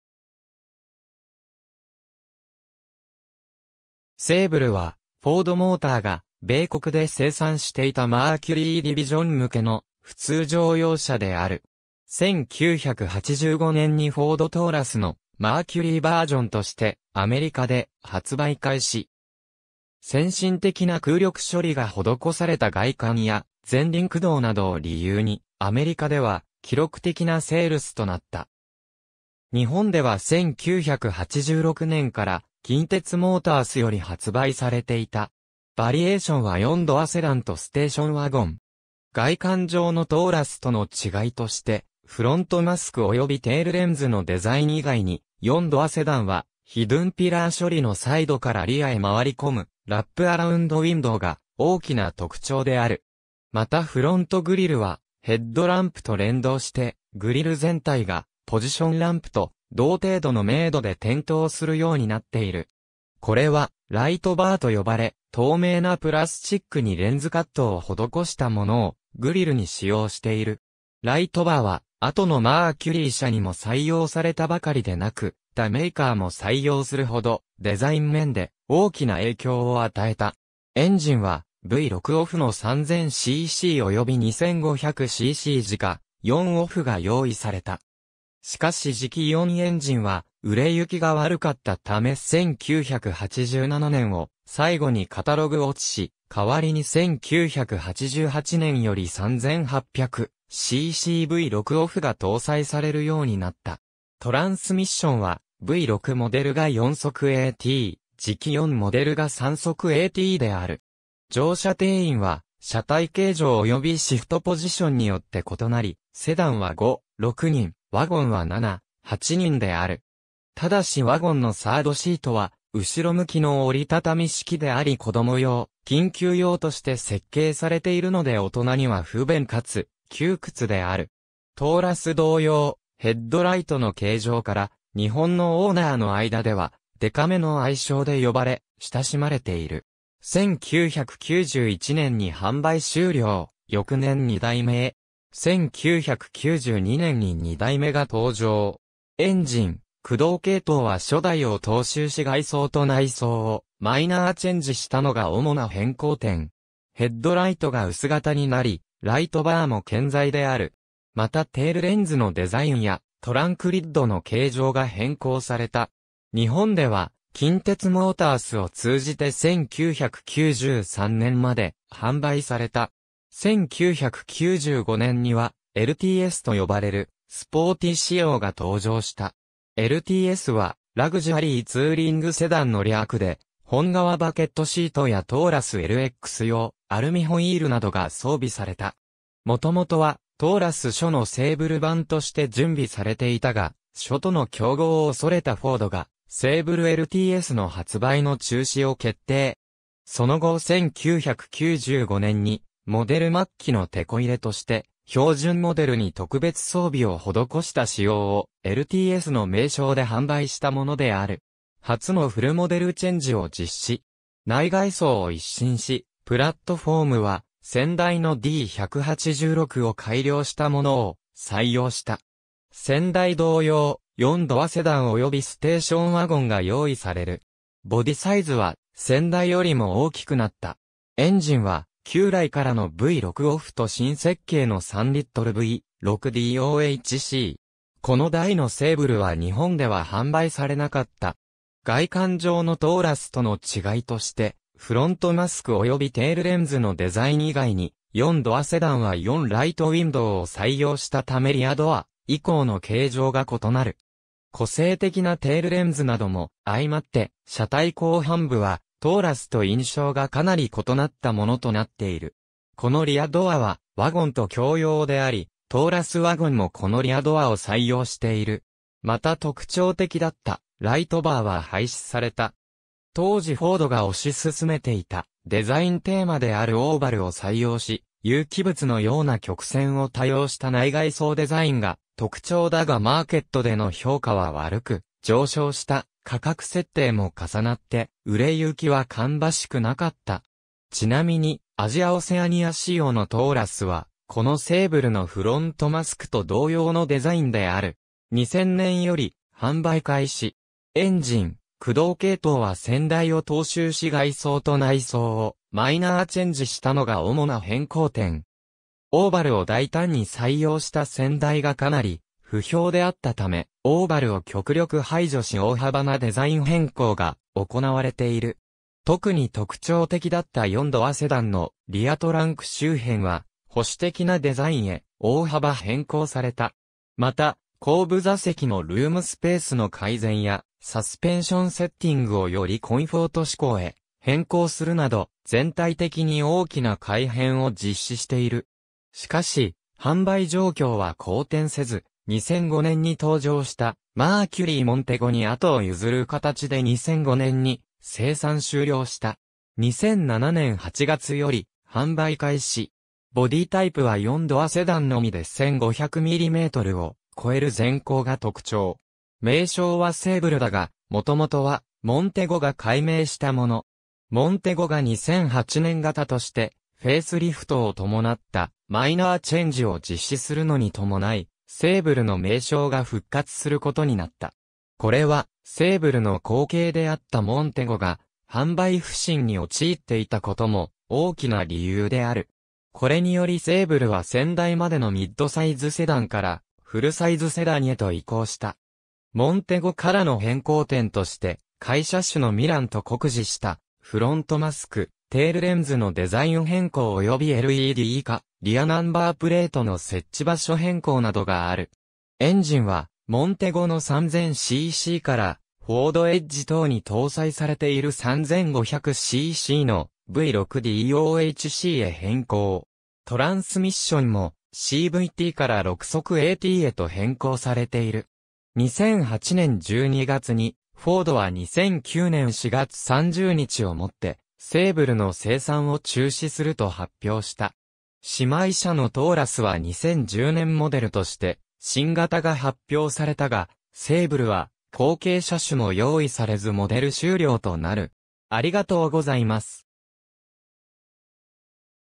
マーキュリー・セーブル
セーブル（Sable）は、フォード・モーターが米国で生産していたマーキュリーディビジョン向けの普通乗用車である。
1985年にフォード・トーラスのマーキュリーバージョンとしてアメリカで発売開始。
先進的な空力処理が施された外観や（CD値はセダン0.33、ワゴン0.35）、前輪駆動などを理由にアメリカでは記録的なセールスとなった。
日本では1986年から近鉄モータースより発売されていた。
バリエーションは4ドアセダンとステーションワゴン。
外観上のトーラスとの違いとしてフロントマスク及びテールレンズのデザイン以外に、4ドアセダンはヒドゥンピラー処理のサイドからリアへ回り込むラップアラウンドウィンドウが大きな特徴である。
またフロントグリルはヘッドランプと連動してグリル全体がポジションランプと同程度の明度で点灯するようになっている。
これはライトバー(lightbar)と呼ばれ、透明なプラスチックにレンズカットを施したものをグリルに使用している。
ライトバーは後のマーキュリー車にも採用されたばかりでなく、他メーカーも採用するほどデザイン面で大きな影響を与えた。
エンジンはV6OHVの3000cc（140ps・22.1kg/m）及び2500cc直4OHV（90ps・18.0kg/m）が用意された。
しかし直4エンジンは売行きが悪かったため1987年を最後にカタログ落ちし、替わりに1988年より3800ccV6OHV（140ps・29.7kg/m）が搭載されるようになった。
トランスミッションはV6モデルが4速AT、直4モデルが3速ATである。
乗車定員は車体形状及びシフトポジションによって異なり、セダンは5/6人、ワゴンは7/8人である。
ただしワゴンのサードシートは後ろ向きの折り畳み式であり子供用・緊急用として設計されているので大人には不便かつ窮屈である。
トーラス同様、ヘッドライトの形状から日本のオーナーの間では「デカ目」の愛称で呼ばれ親しまれている。
1991年に販売終了、翌年2代目へ。
1992年に2代目が登場。
エンジン・駆動系統は初代を踏襲し外装と内装をマイナーチェンジしたのが主な変更点。
ヘッドライトが薄型になり、 ライトバーも健在である。
またテールレンズのデザインやトランクリッドの形状が変更された。
日本では近鉄モータースを通じて1993年まで販売された。
1995年にはLTSと呼ばれるスポーティ仕様が登場した。
LTSはLuxury Touring Sedanの略で、本革バケットシートやトーラスLX用アルミホイールなどが装備された。
元々はトーラスSHOのセーブル版として準備されていたが、SHOとの競合を恐れたフォードがセーブルLTSの発売の中止を決定。
その後1995年にモデル末期のテコ入れとして標準モデルに特別装備を施した仕様をLTSの名称で販売したものである。
初のフルモデルチェンジを実施。
内外装を一新し、プラットフォームは先代のD186を改良したものを採用した。
先代同様、4ドアセダン及びステーションワゴンが用意される。
ボディサイズは先代よりも大きくなった。
エンジンは旧来からのV6OHVと新設計の3L・V6DOHC。
この代のセーブルは日本では販売されなかった。
外観上のトーラスとの違いとして、フロントマスク及びテールレンズのデザイン以外に、4ドアセダンは4ライトウィンドウを採用したためリアドア以降の形状が異なる。
個性的なテールレンズなども相まって、車体後半部はトーラスと印象がかなり異なったものとなっている。
このリアドアはワゴンと共用であり、トーラス・ワゴンもこのリアドアを採用している。
また特徴的だったライトバーは廃止された。
当時フォードが推し進めていたデザインテーマであるオーバルを採用し、有機物の様な曲線を多用した内外装デザインが特徴だがマーケットでの評価は悪く、上昇した価格設定も重なって売行きは芳しくなかった。
ちなみにアジア・オセアニア仕様のトーラスは、このセーブルのフロントマスクと同様のデザインである。
2000年より販売開始。
エンジン・駆動系統は先代を踏襲し外装と内装をマイナーチェンジしたのが主な変更点。
オーバルを大胆に採用した先代がかなり不評であったため、オーバルを極力排除し大幅なデザイン変更が行われている。
特に特徴的だった4ドアセダンのリアトランク周辺は保守的なデザインへ大幅変更された。
また後部座席のルームスペースの改善やサスペンションセッティングをよりコンフォート...